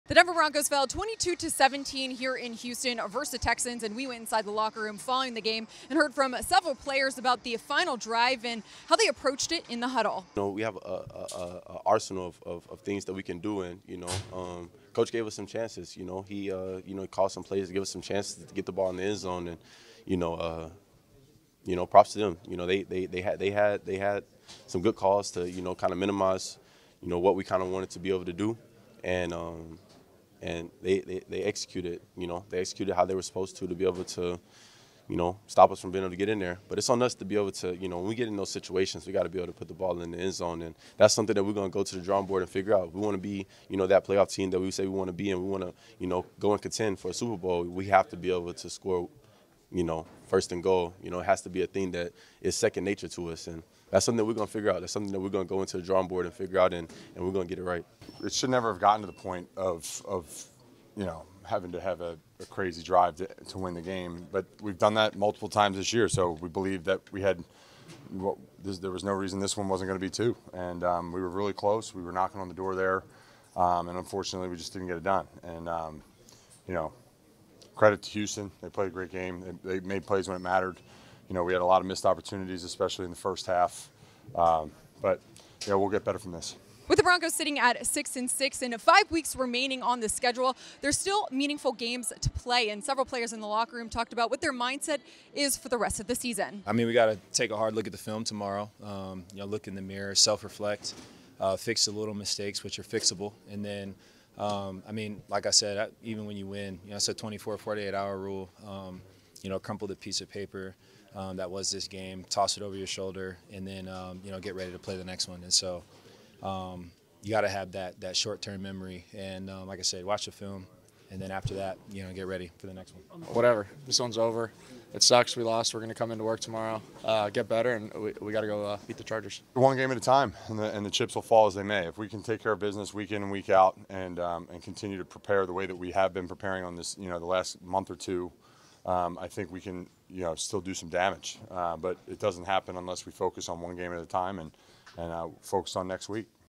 Inside the Locker Room: Broncos react to loss in Houston
Team reporter Sydney Jones goes inside the locker room at NRG Stadium in Houston to detail the Broncos' final drive of the game and how the team moves past the loss.